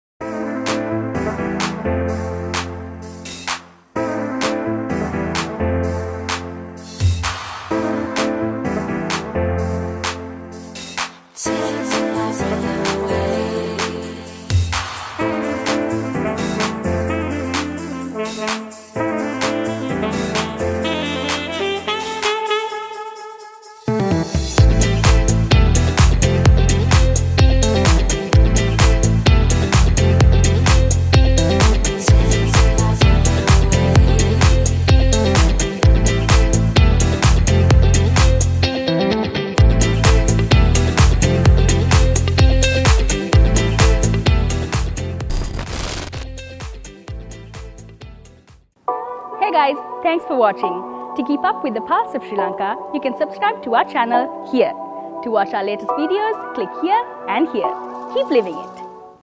Novid-20 Presented by the Redeemers
An evening of musical celebration by the Redeemers unfolded on the 5th of September at Cinnamon Lakeside Colombo, and here's everything we captured from the wonderful event!